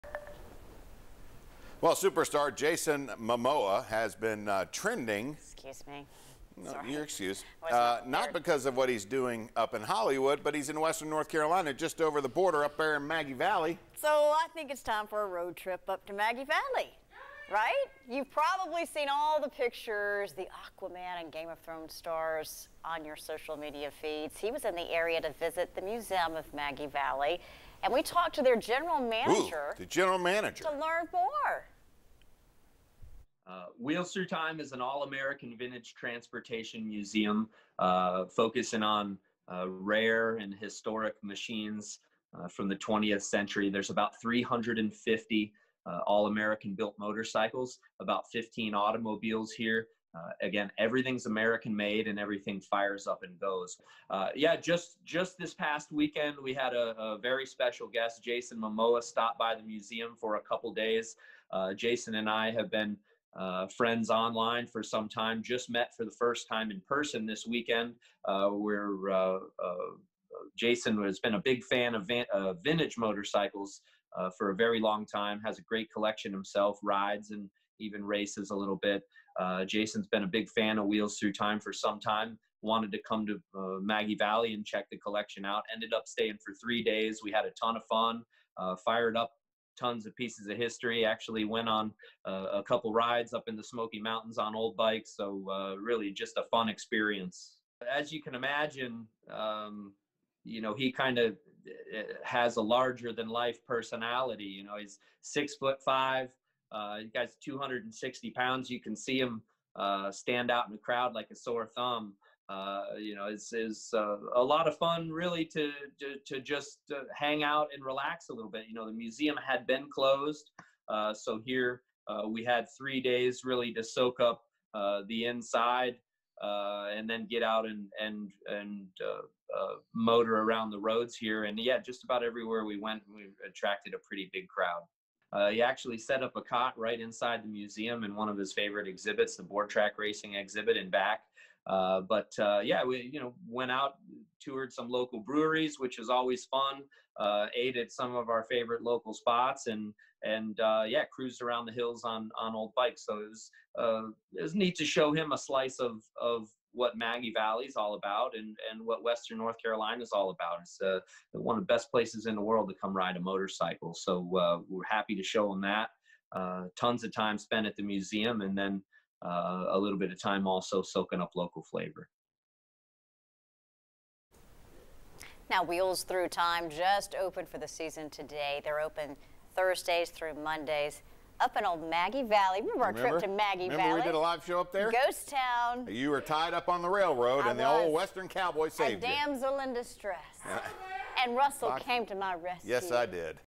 Jason Momoa visits Maggie Valley Motorcyle Museum
Actor Jason Momoa is a motorcycle enthusiast. Recently, he made a visit to the Wheels Through Time museum in Maggie Valley, NC, wheelsthroughtime.com. 10/01/2020-4pm